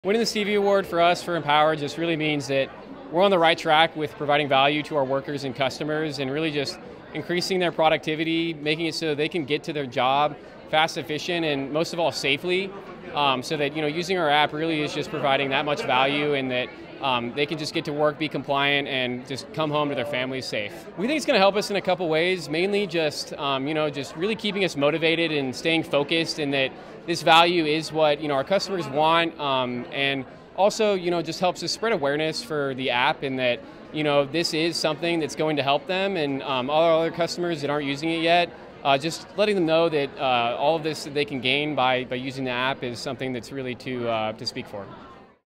ISN is a Stevie® Award Winner in The 2024 American Business Awards®
The 22nd edition of The American Business Awards®, the top honors in the USA for achievement in the workplace, were presented on June 11 in New York. The American Business Awards® are open to all organizations operating in the U.S.A., and feature a wide variety of categories to recognize achievement in every facet of the workplace.